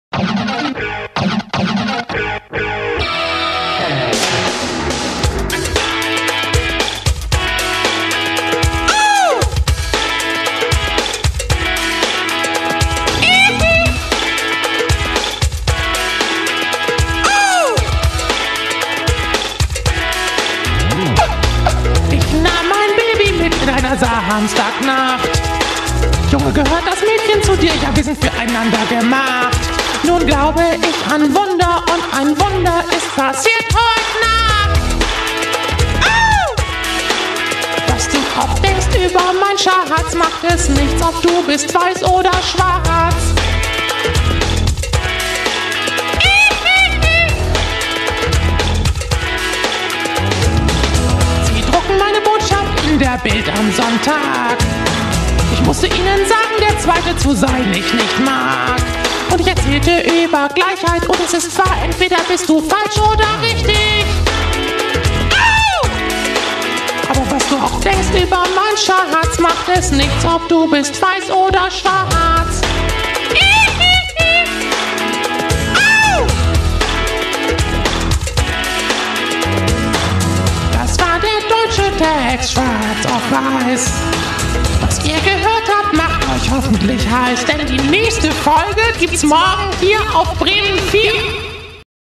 Teil 4: Arnie Jackson
Tuddi präsentiert das Fanvideo 2008/2009 !!! Deutschmacher Arnd Zeigler trifft musikalisch auf Stadionsprecher Arnie.